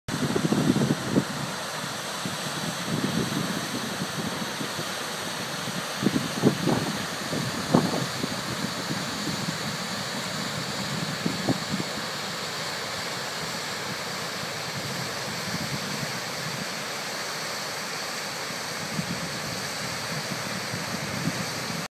Dynjandi Waterfalls
On the way from Hellnar to Isafjördur, the Dynjandi waterfalls was a nice quiet rest stop. This was taken a week before tourist season started.